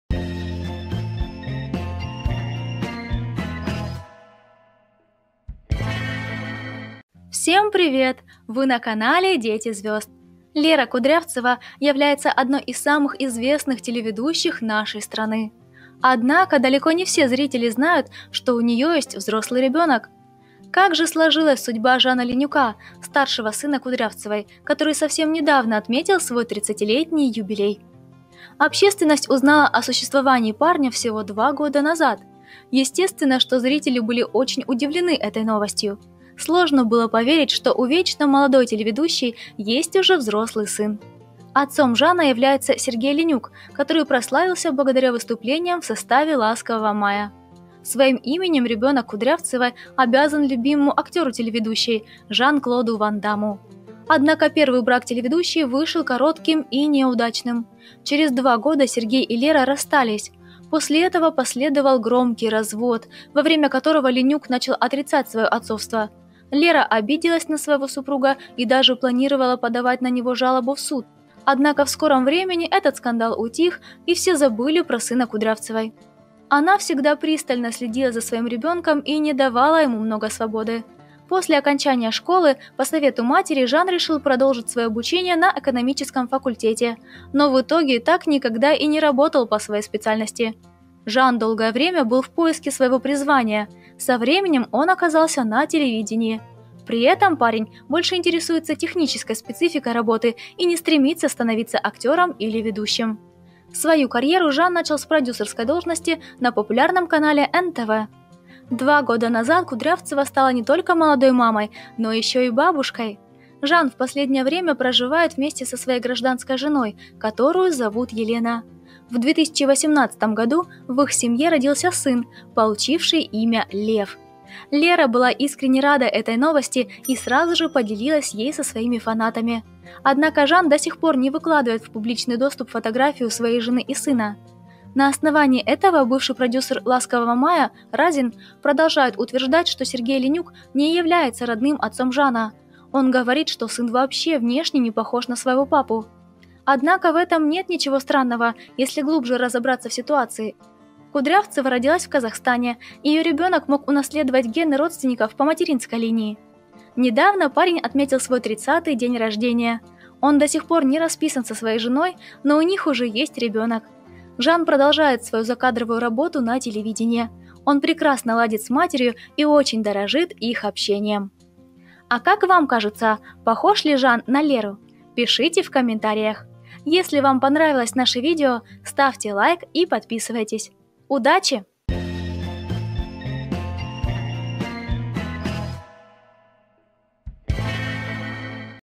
Как сложилась судьба у сына Леры Кудрявцевой и почему он внешне похож на казаха
Лера Кудрявцева является одной из самых известных телеведущих нашей страны. Однако далеко не все зрители знают, что у нее есть взрослый ребенок. Как же сложилась судьба Жана Ленюка, старшего сына Кудрявцевой, которой совсем недавно отметил свой тридцатилетний юбилей.
Общественность узнала о существовании парня всего 2 года назад. Естественно, что зрители были очень удивлены этой новостью. Сложно было поверить, что у вечно молодой телеведущей есть уже взрослый сын. Отцом Жана является Сергей Ленюк, который прославился благодаря выступлениям в составе «Ласкового мая».
Своим именем ребенок Кудрявцевой обязан любимому актеру телеведущей – Жан-Клоду ван Дамму. Однако первый брак телеведущей вышел коротким и неудачным. Через 2 года Сергей и Лера расстались. После этого последовал громкий развод во время, которого Ленюк начал отрицать свое отцовство. Лера обиделась на своего супруга и даже планировала подавать на него жалобу в суд. Однако в скором времени этот скандал утих и все забыли, про сына Кудрявцевой.